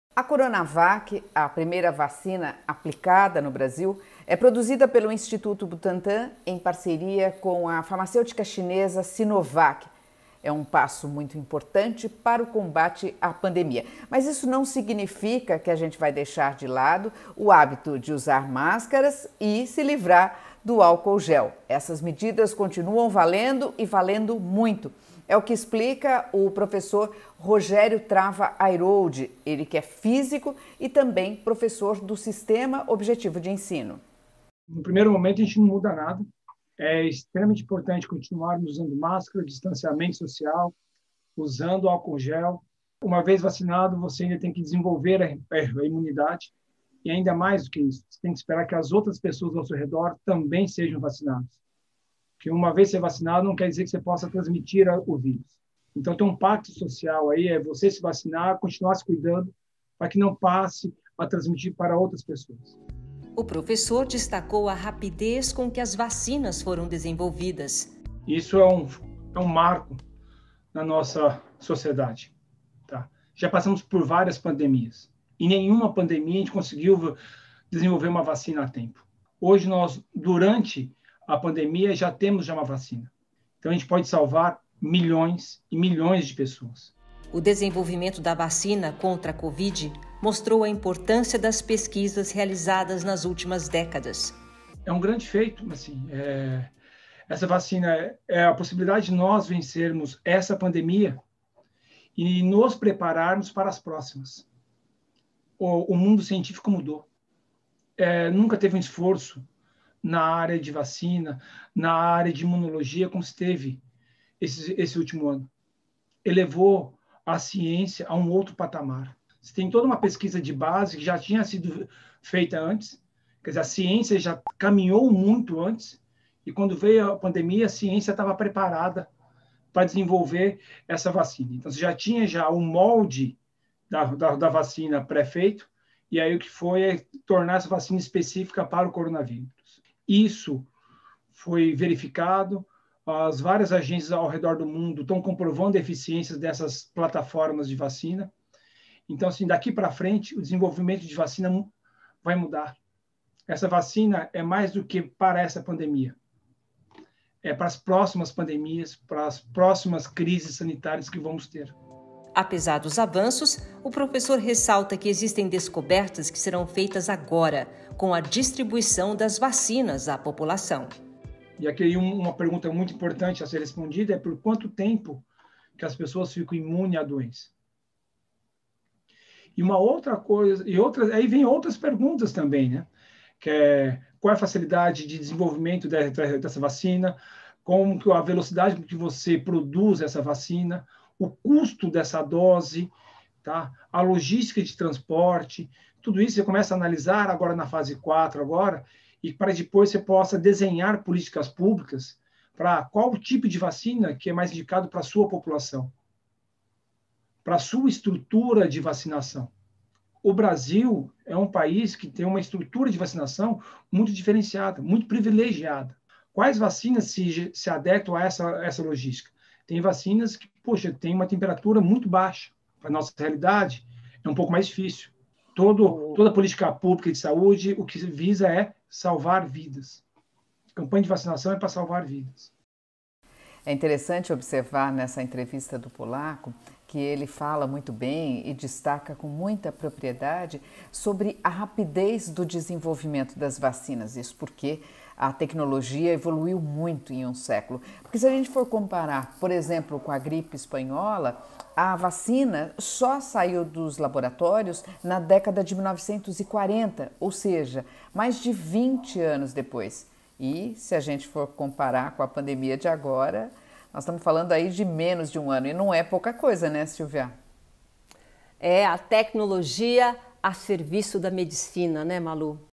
VACINA NÃO EXCLUI MEDIDAS PREVENTIVAS
Na matéria de hoje, o físico Rogério Airoldi destaca a importância de manter as medidas preventivas mesmo com a chegada da vacina.

Muito obrigada pela participação 😉 @rogeriotravaairoldi

👥 EQUIPE
Apresentadoras: @jornalista.malumota e @silvia_vinhas
Editores de imagem: @rauldjacomini e @schmoozah
Editores de texto: @cristobace e Fabricio Presilli
Estagiárias: @anitaherog @amandasaccucci @gabpastorelli